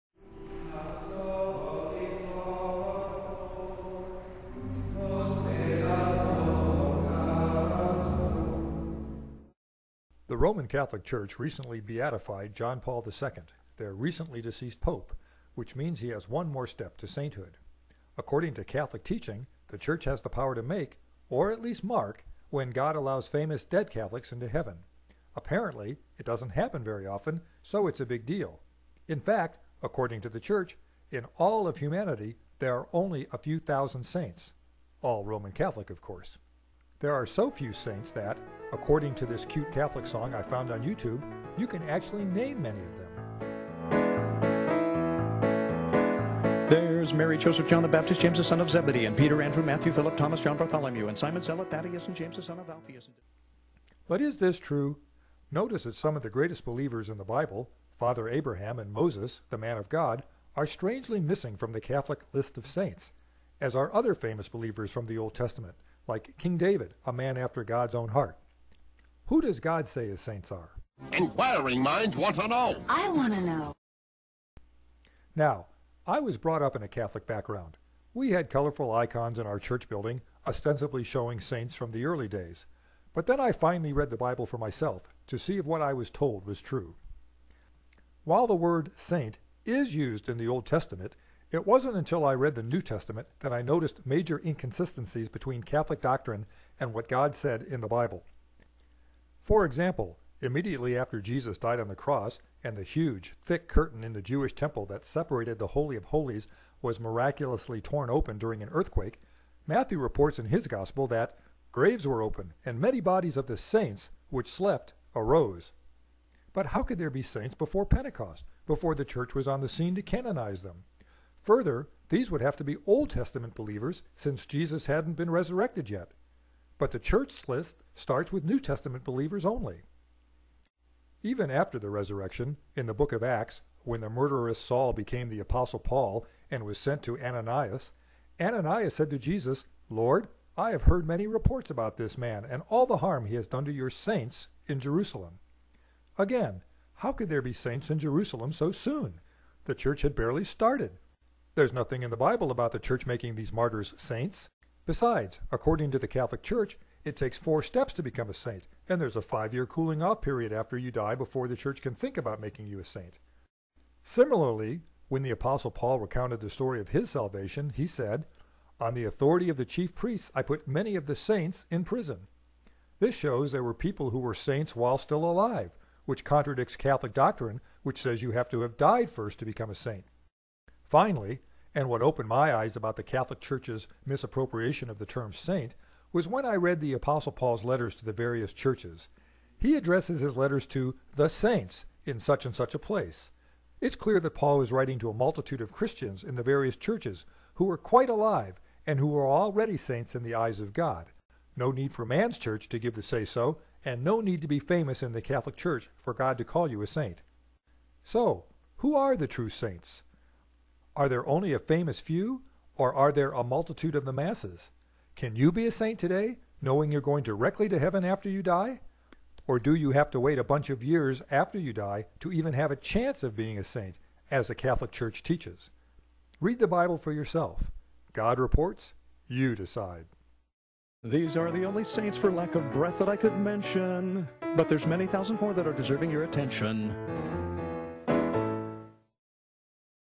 Ain't no (Catholic) Saint
If you're Catholic, even your own church says your odds of becoming a saint are nil. Wouldn't you rather be a Christian?